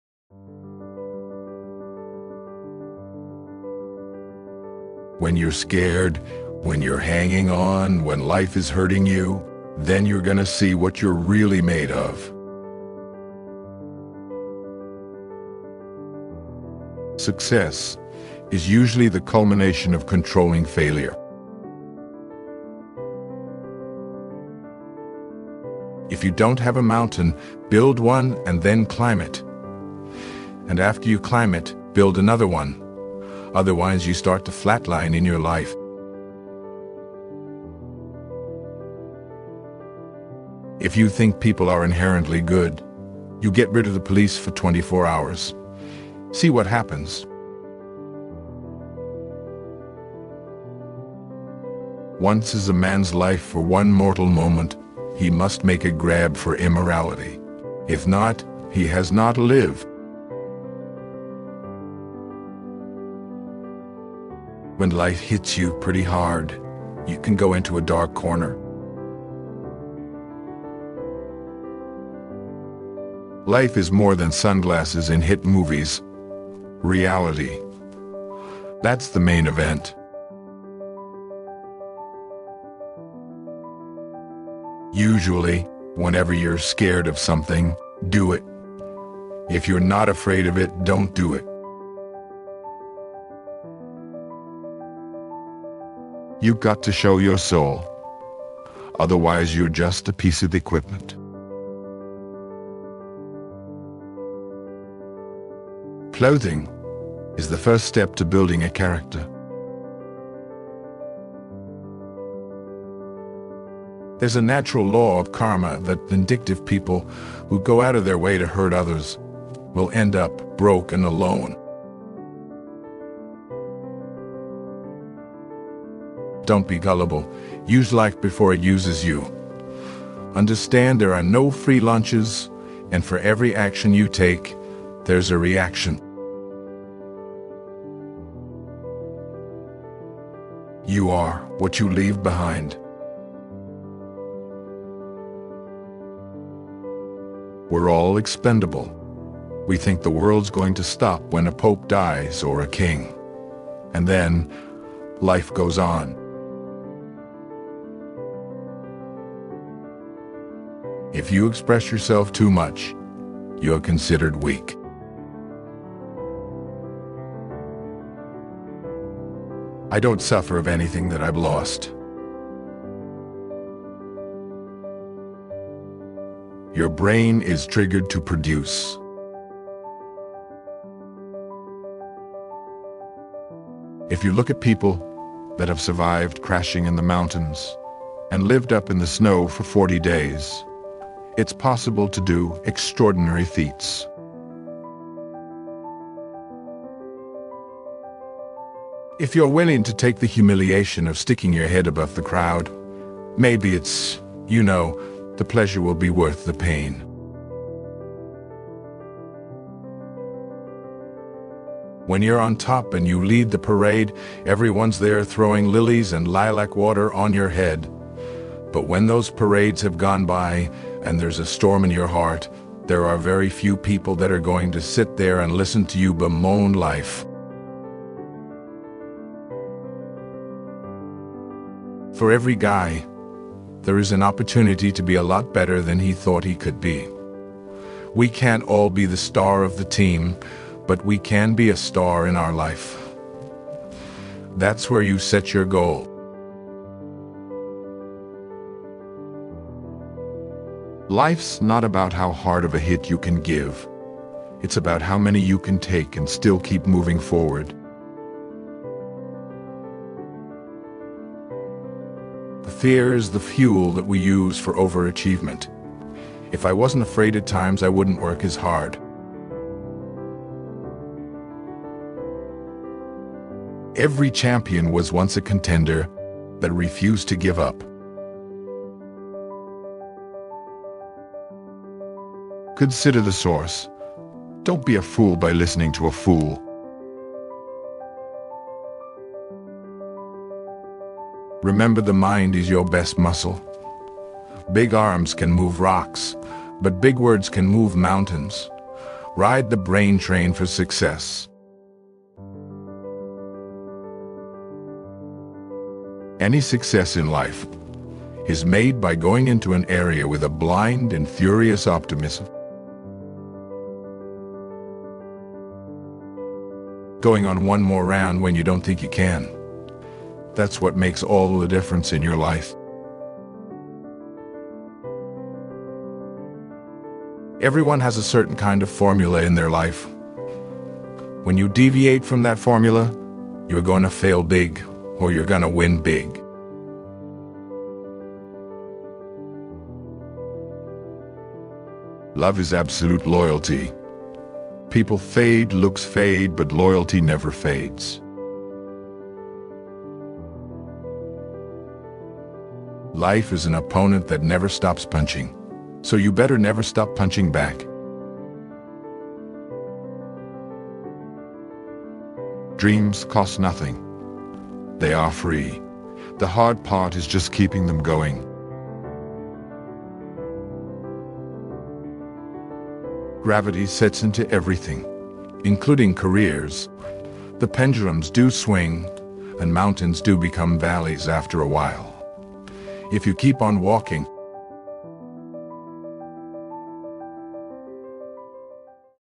The Art of Never Giving Up: Sylvester Stallone's Most Powerful Quotes
Sylvester Stallone, an American actor and filmmaker, is known for his iconic roles in the box-office movie series, such as 'Rocky' and 'Rambo'. After years of being a struggling actor in New York City, he is the living proof that hard work and dedication will get you where you want to be.

TopQuotes is a channel dedicated to provide inspirational quotes from some of the most world-renowned figures to motivate you.